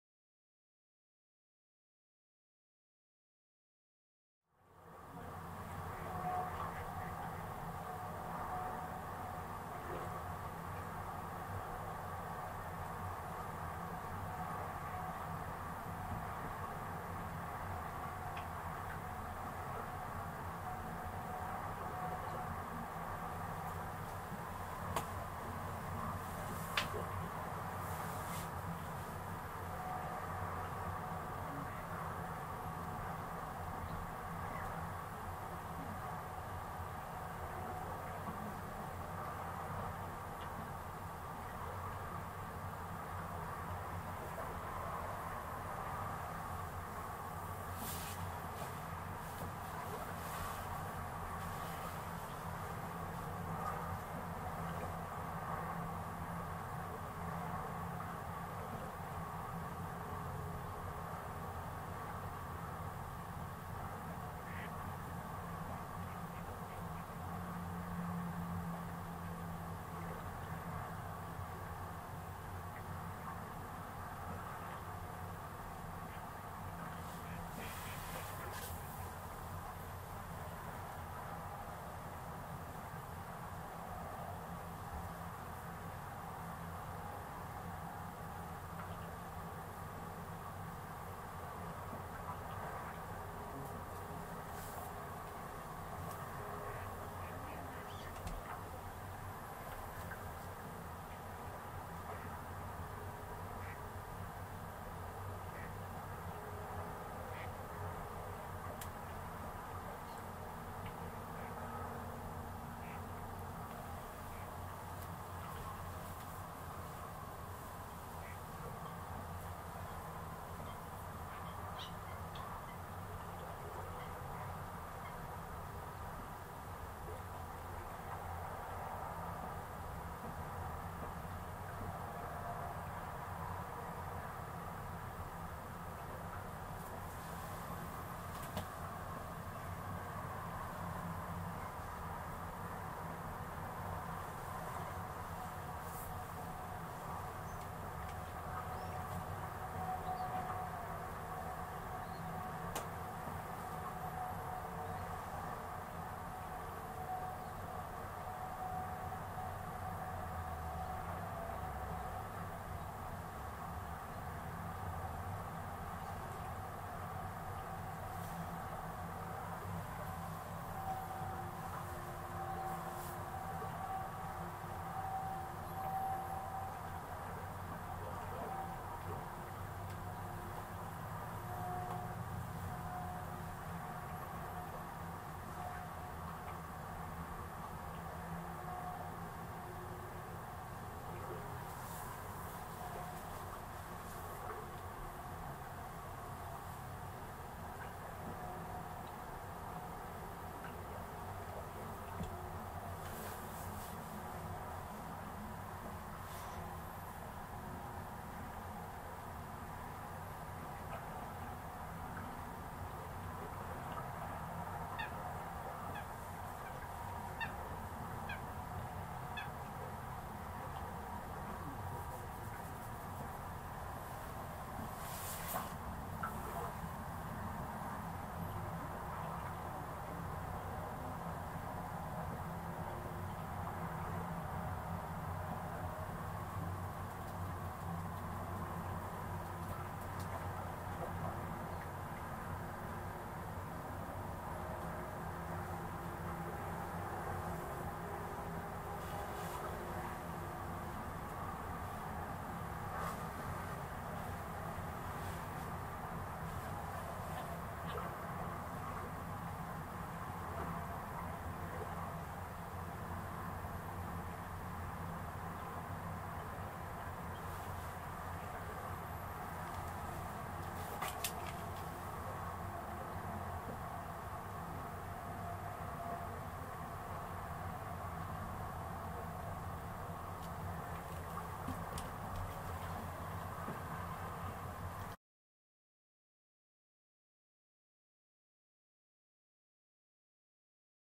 Entspannung am Jungernweiher - wer hat noch mehr Geduld
Unterwegs überwiegend in der Eifel und an der Mosel -
kommen Sie mit mir um die Natur zu genießen -

heute, ein entspannter Besuch am Jungfernweiher in Ulmen in der Eifel.